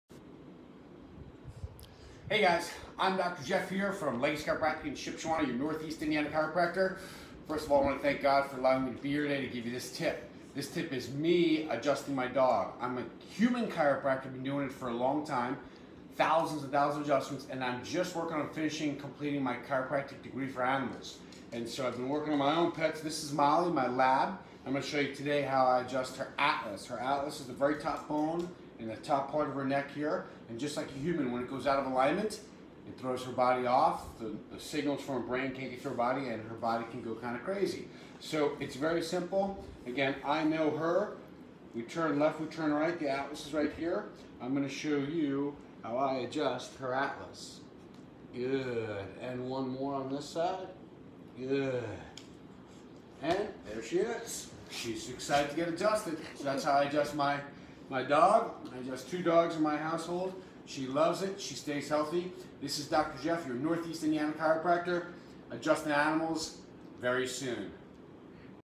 Dog Neck Adjustment
Animals need Chiropractic Just like humans.

Watch as I keep my Dog healthy by Adjusting his upper Neck called the Atlas.

I adjust my dogs regularly to kerp them healthy and moving like they were born to do!

I am almost done with my 240 hour animal chiropractic certification through Animal Chiropractic Education Source or ACES in Texas!

2 Labs to go!